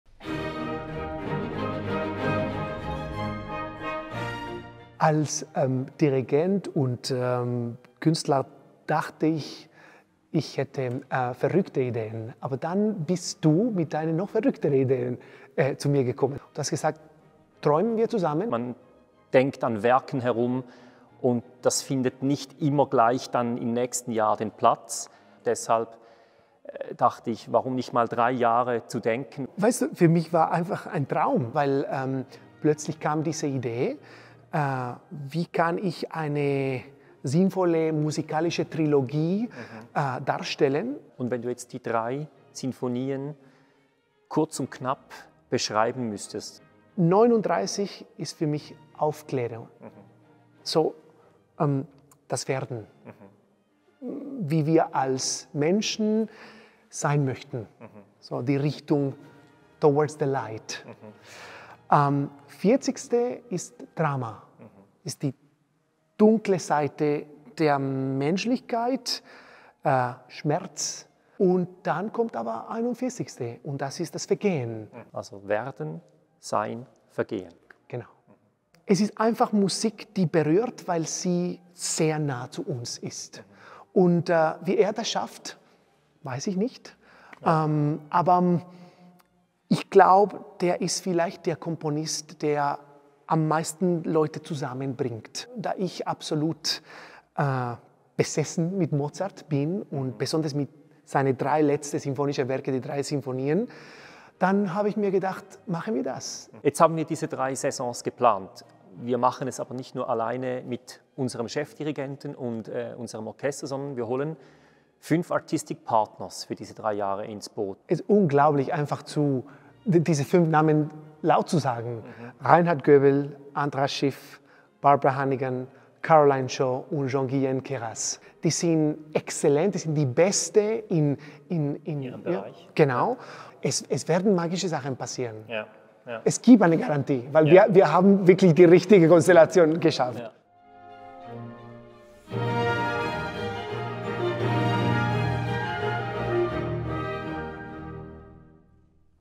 Saison 2022/23 - Trailer
Mozarts Sinfonien spannen den Bogen über die nächsten drei Saisons des Musikkollegiums Winterthur. Sie bilden den Kern einer Trilogie, die unter dem Titel WERDEN – SEIN – VERGEHEN den Lebenszyklus symbolisiert. Neu sind auch die beiden Formate FOLLOW und Artistic Partners, die dem Publikum neue Impulse bieten und spannende Begegnungen mit Künstlerinnen und Künstlern ermöglichen.

Alle Informationen finden Sie unter musikkollegium.ch